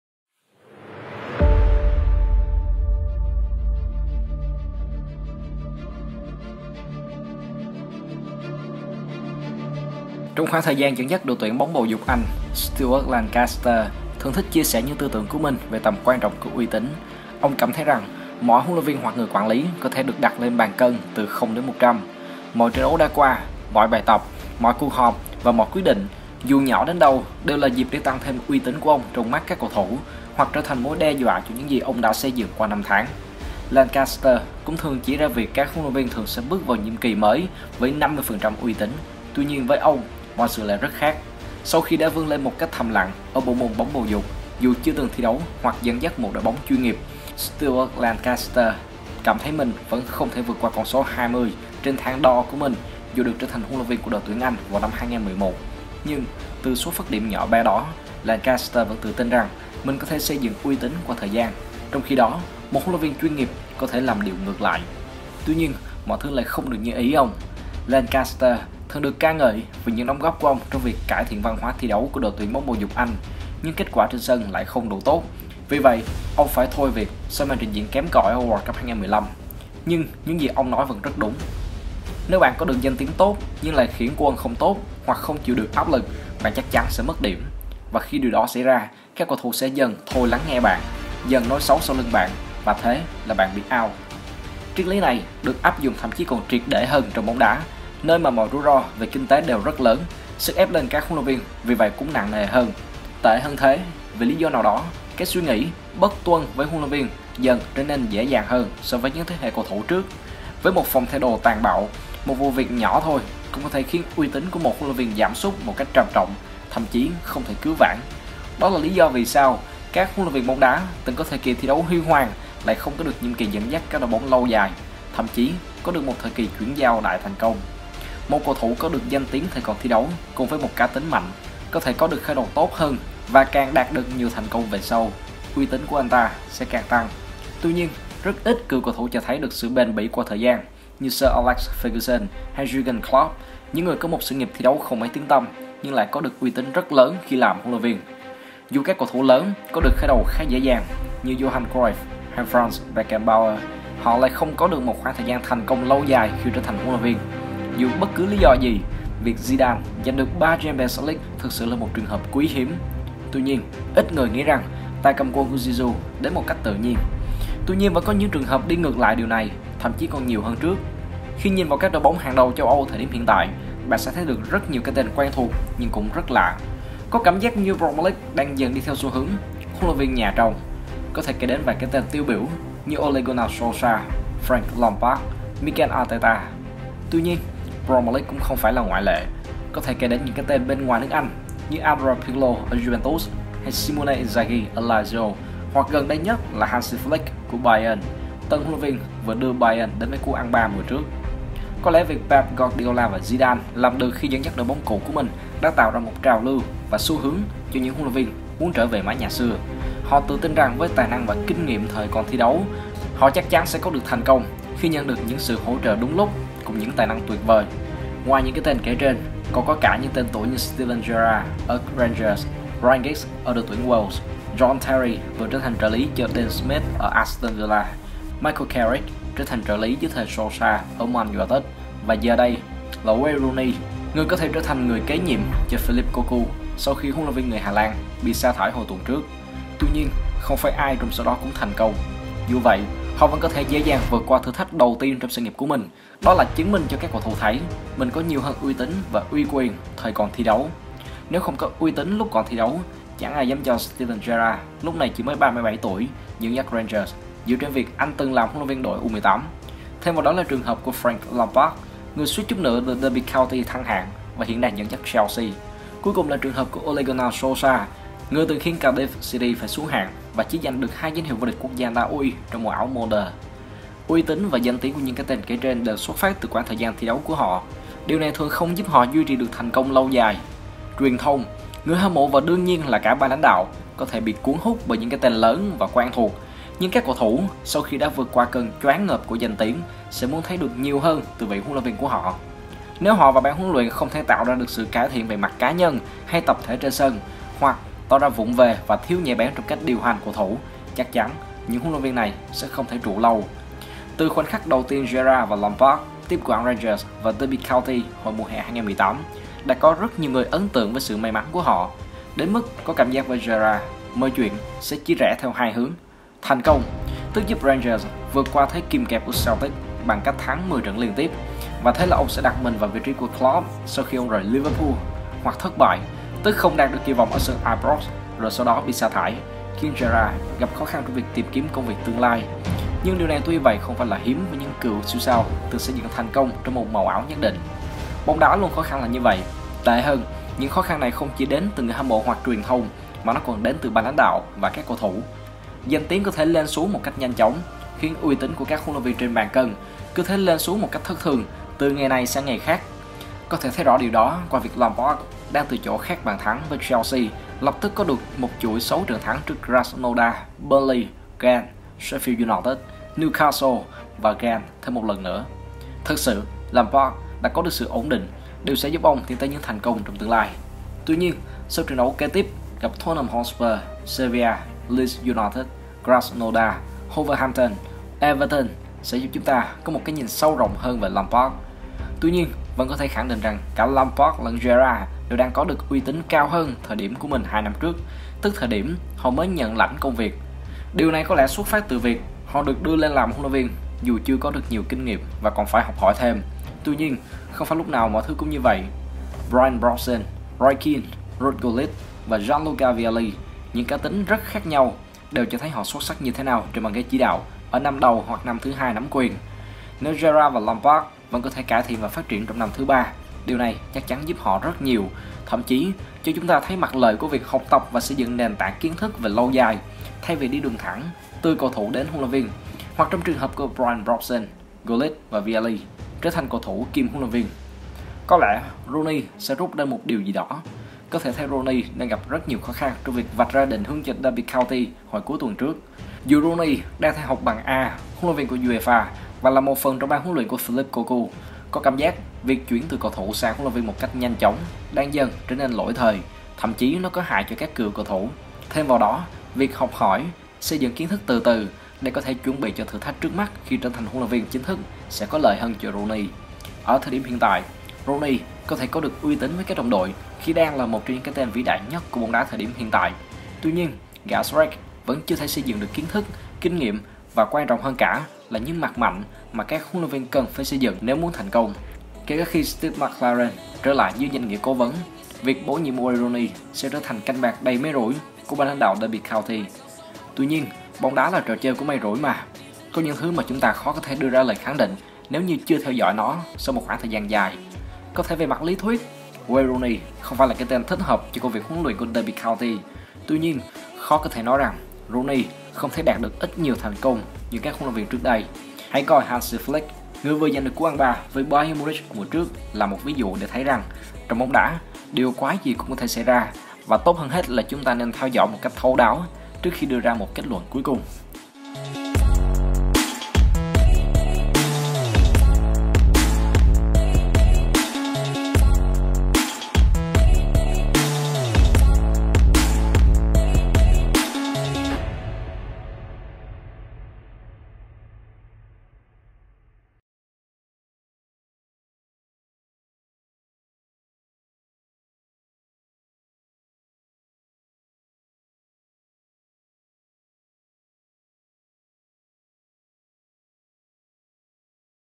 WAYNE ROONEY liệu có thích hợp để trở thành HUẤN LUYỆN VIÊN của DERBY COUNTY?
Có thể về mặt lý thuyết Rooney không phải là cái tên thích hợp cho công việc huấn luyện của Derby County. Tuy nhiên, khó có thể nói rằng Rooney không thể đạt được ít nhiều thành công như các HLV trước đây. Hãy coi Hansi Flick, người vừa giành được cú ăn 3 với Bayern Munchen ở mùa trước làm một ví dụ để thấy rằng, trong bóng đá điều gì cũng có thể xảy ra.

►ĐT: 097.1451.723